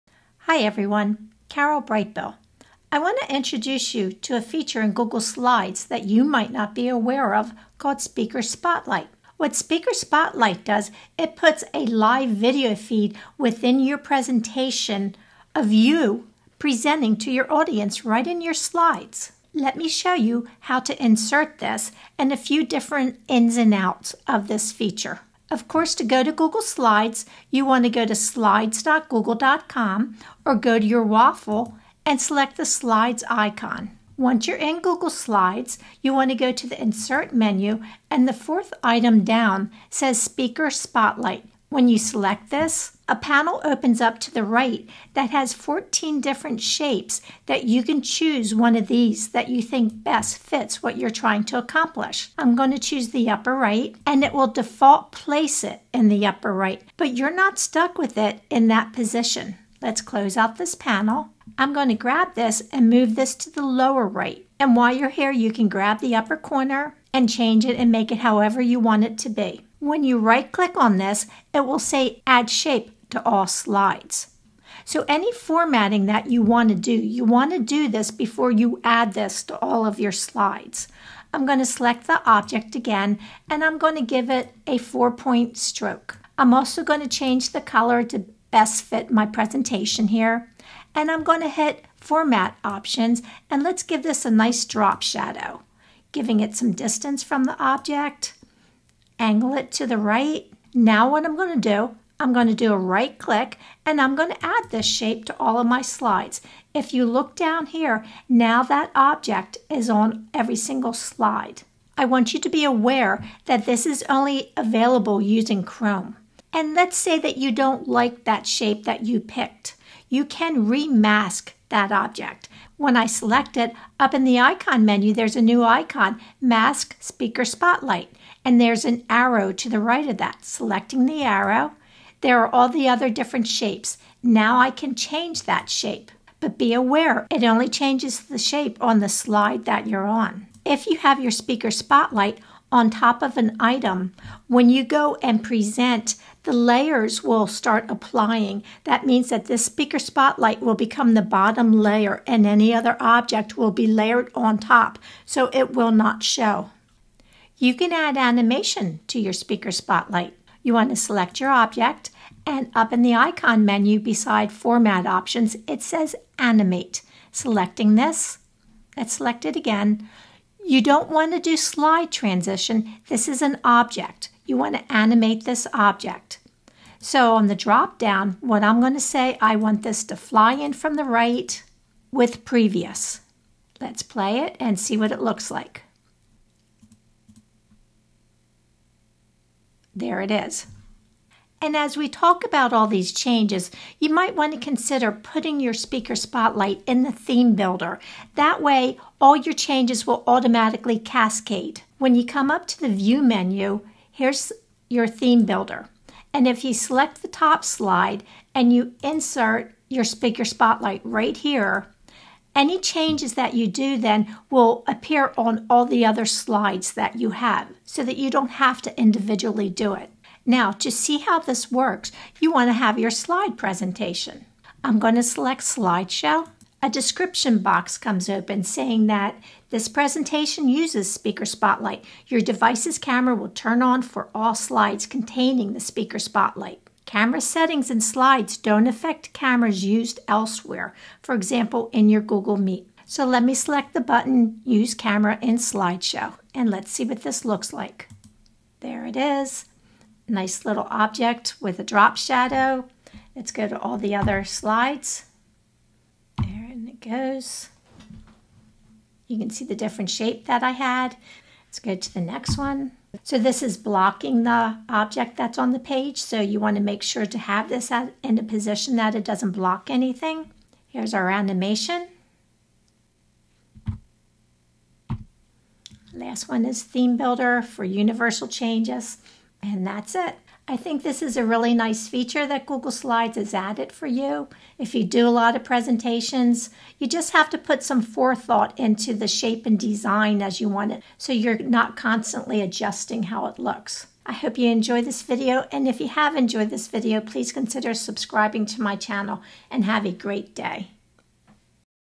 How to have a live video feed of you presenting right in Google Slides called Speaker Spotlight!
WORKSPACE ACCOUNT ONLY...
How to have a live video feed of you presenting right in Google Slides called Speaker Spotlight! This adds your video feed right inside of your presentation. It doesn't have to be on every slide, just the ones. you choose.